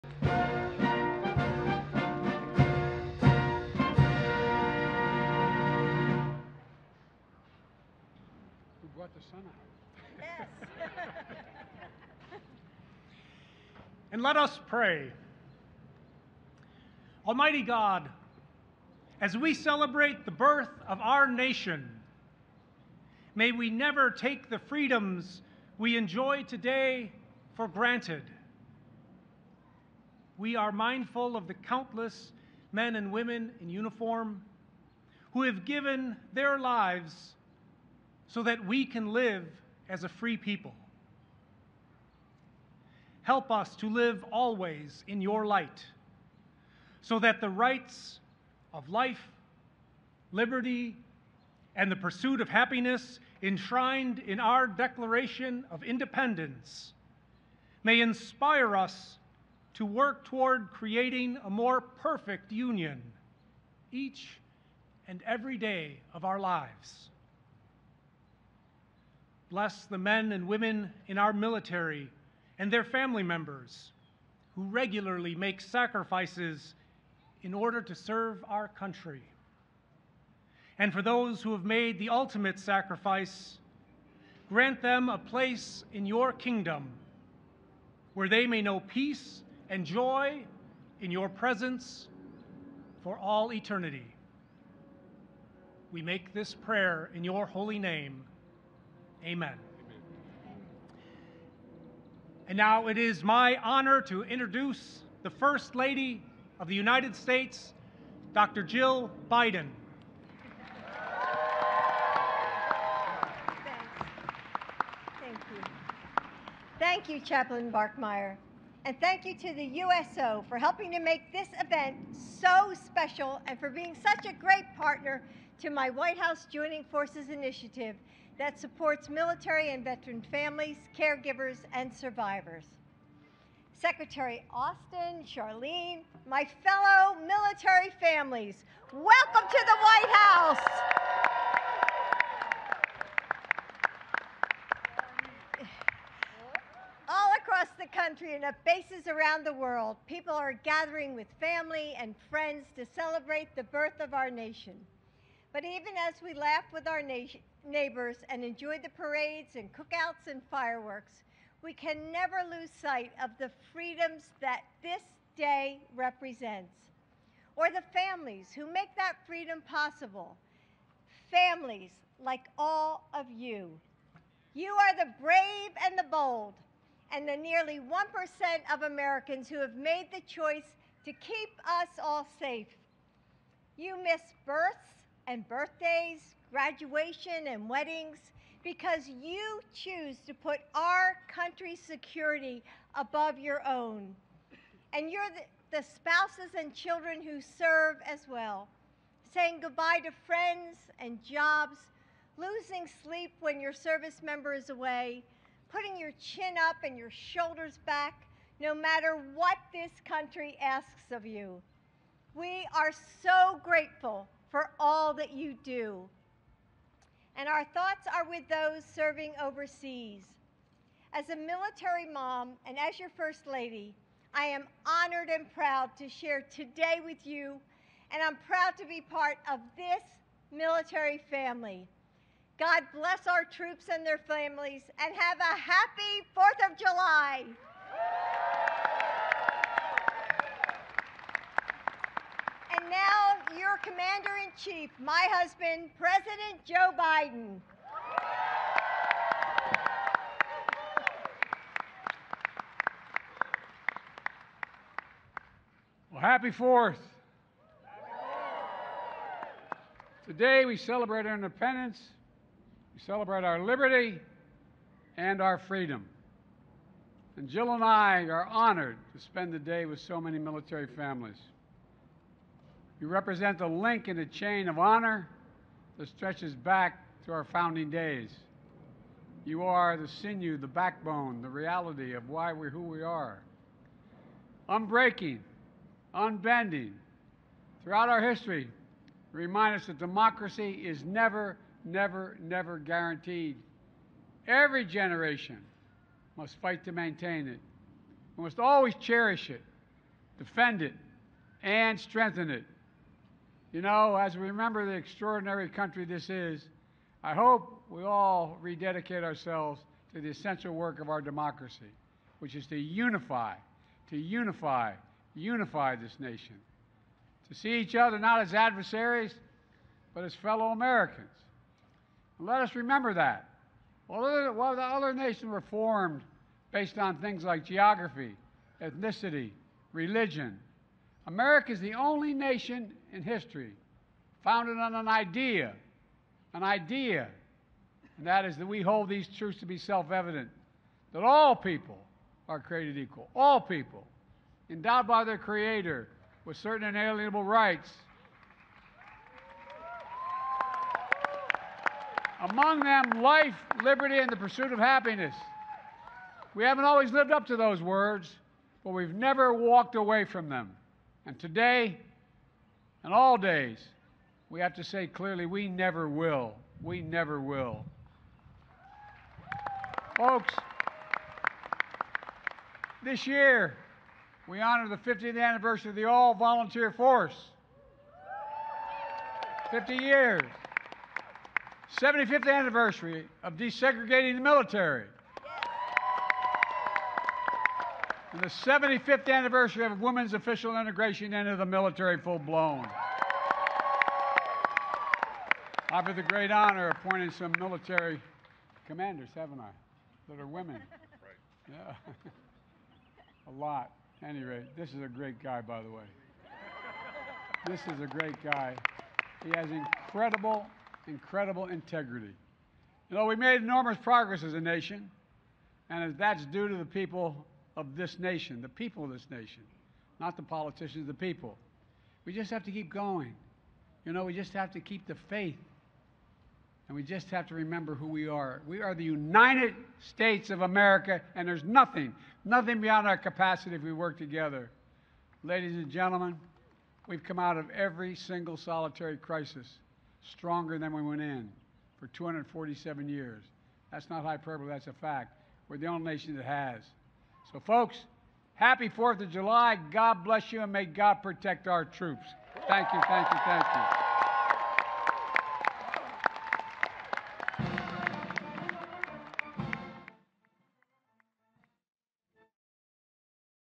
BREAKING NEWS: Biden Holds Independence Day Event For Military Service-Members At White House
President Biden holds an Independence Day event at the White House for military and National Guard service-members and their families.